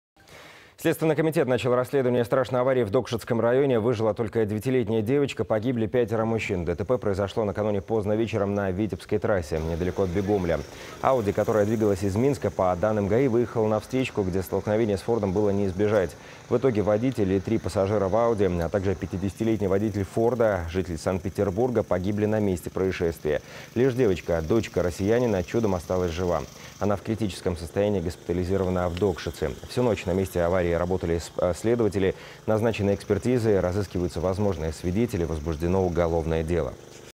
Подробности ДТП, произошедшей на витебской трассе
Следственный комитет уже начал расследование, возбуждено уголовное дело.